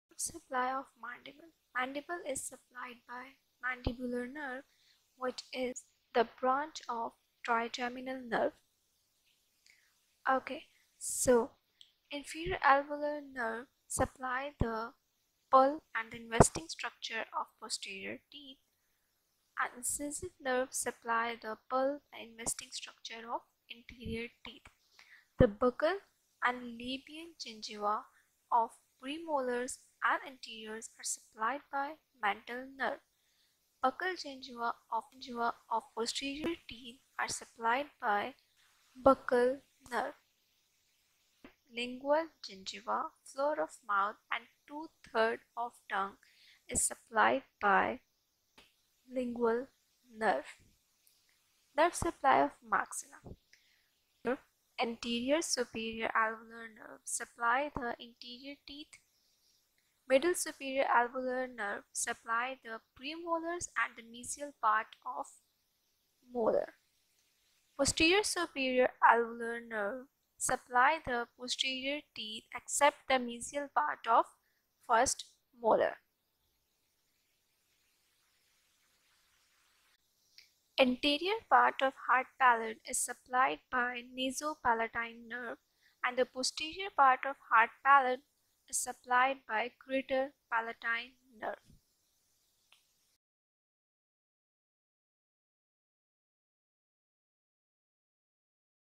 NERVE SUPPLY / INNERVATION OF MAXILLA AND MANDIBLE
In this video, I am explaining the nerve supply of mandibular and maxillary teeth and its surrounding structure in a very precise and quick form.
Mandibular nerve and its supply to the mandible.
Maxillary nerve and its supply to the maxilla.

0:00 - Nerve Supply Of Mandible
0:11 - Inferior Alveolar Nerve
0:20 - Incisive nerve
0:25 - Mental Nerve
0:35 - Buccal Nerve
0:41 - Lingual Nerve
0:52 - Nerve Supply Of Maxilla
0:55 - Anterior superior alveolar nerve block
1:00 - Middle superior alveolar nerve block
1:00 - Posterior superior alveolar nerve block
1:21 - Nasopalatine nerve block
1:29 - Greater Palatine nerve block